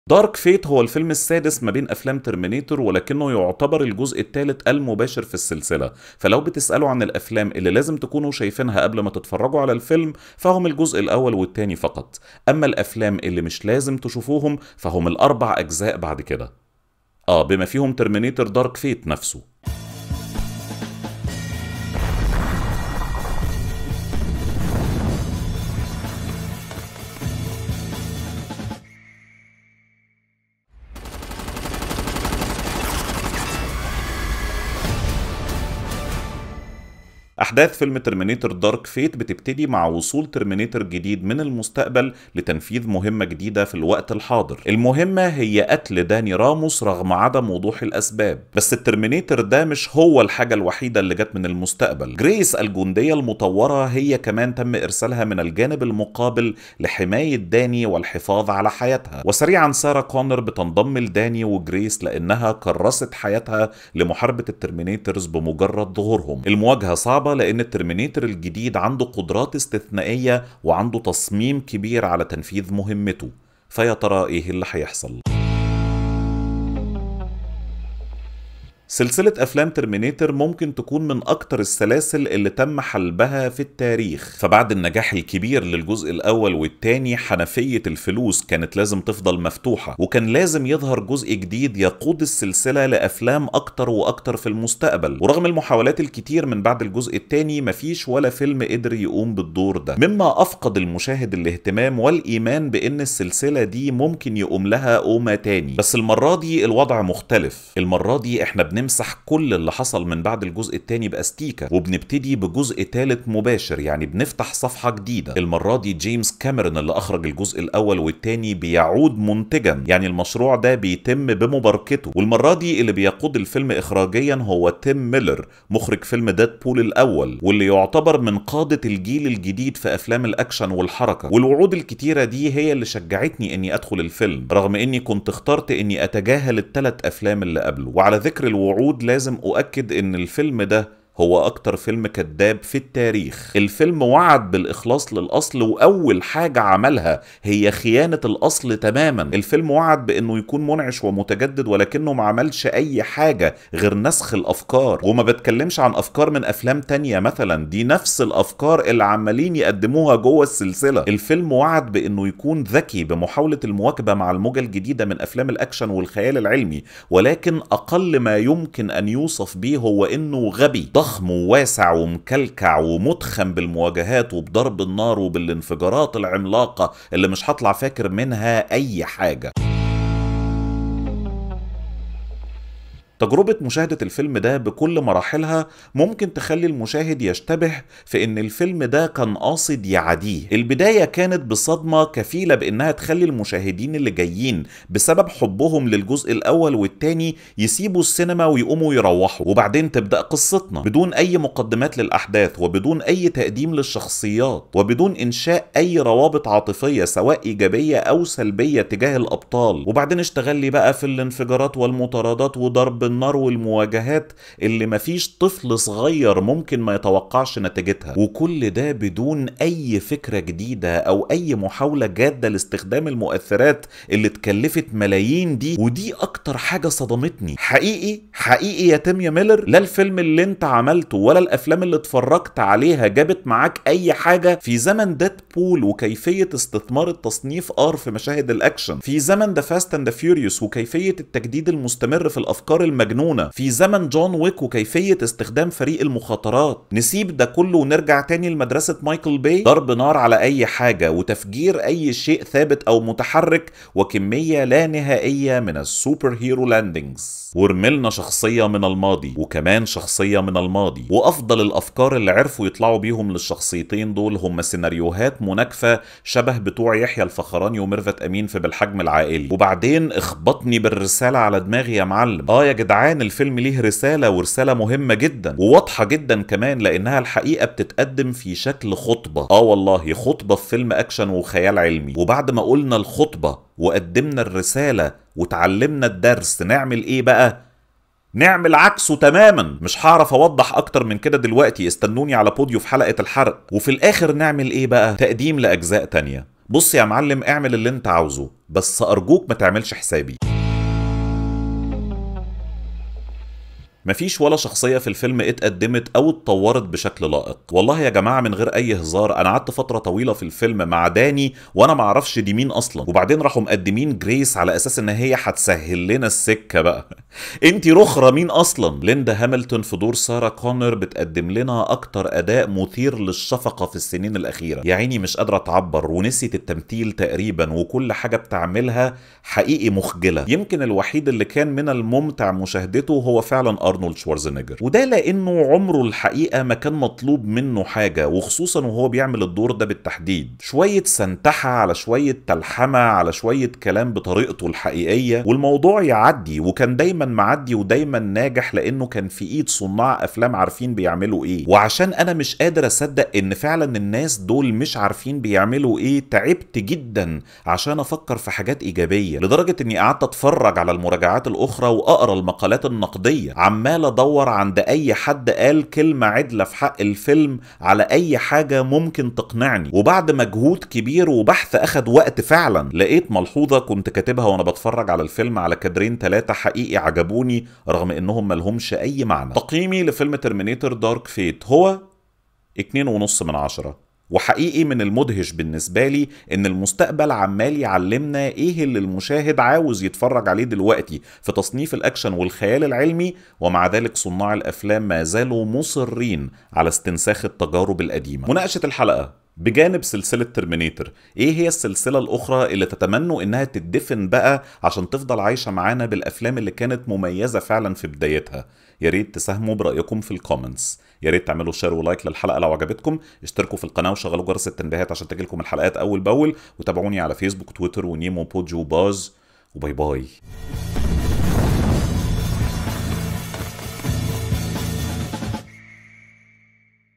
مراجعة فيلم "Terminator: Dark Fate" بالعربي | FilmGamed
مراجعة فيلم Terminator: Dark Fate من فيلم_جامد. الفيلم هو السادس بين سلسلة أفلام Terminator ولكنه يعتبر الجزء الثالث المباشر على مستوى الأحداث. الفيلم من بطولة ليند هاملتون وأرنولد شوارزنيجر وماكينزي ديفيس وجابرييل لونا وناتاليا رييس. السيناريو لديفيد إس جوير وجاستن رودز وبيلي راي والإخراج لتيم ميلر.